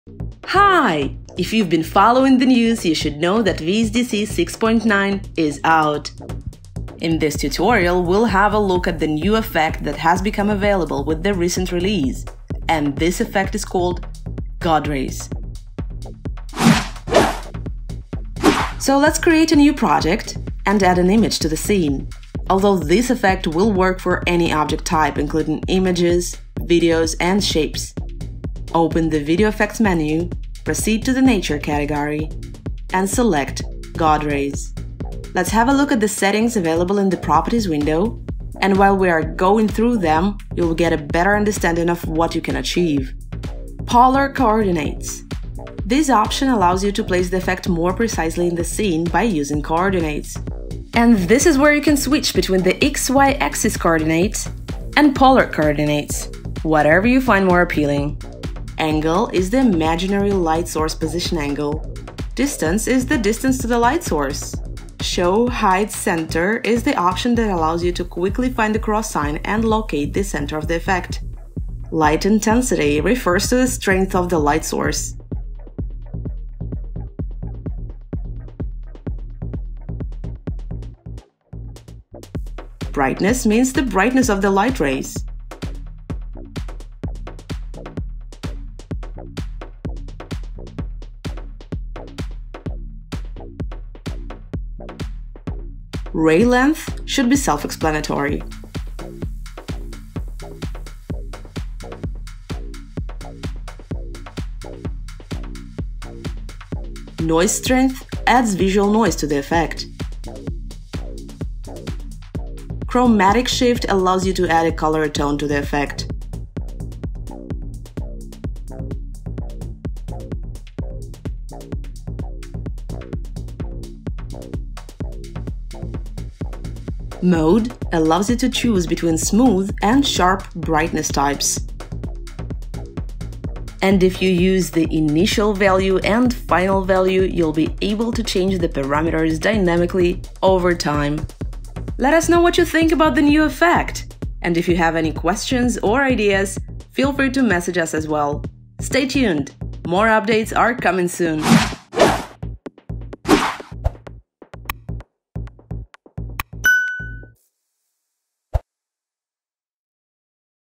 How to make God rays effect in VSDC Video Editor Pro
Have you downloaded the new version of VSDC already?
Well, we’re about to give you a good reason to hurry up and do so

Among other cool features, VSDC 6.9 brings a dramatic effect, called God rays. You can apply it to images, videos, text titles, and shapes to achieve the illusion of light rays piercing through the gaps of the imaginary atmosphere or object’s shape.

Time stamps:
00:00 Intro
00:20 Add an object to the scene
00:30 Apply the God rays effect
00:41 Settings overview
00:43 "Polar coordinates"
00:58 "Angle"
01:02 "Distance"
01:05 "Show/hide center"
01:11 "Light intensity"
01:22 "Brightness"
01:36 "Ray length"
01:47 "Noise strength"
01:53 "Chromatic shift"
02:06 "Brightness mode"
02:13 Dynamic parameter change
02:21 Outro